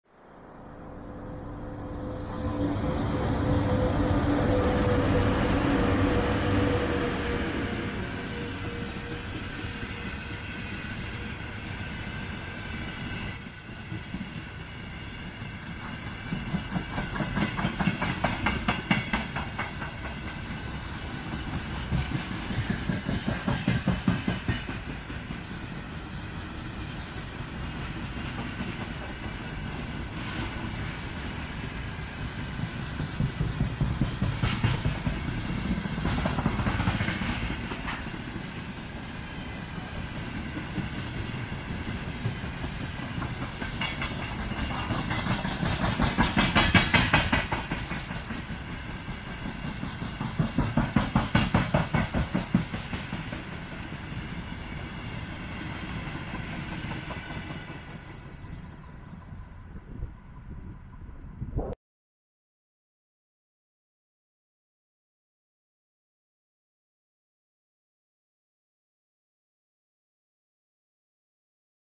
Cargo train consisting only of green Gondola cars (4K)
Cargo two-section AC electric locomotive VL80 pulls only green gondola cars. 4K quality video.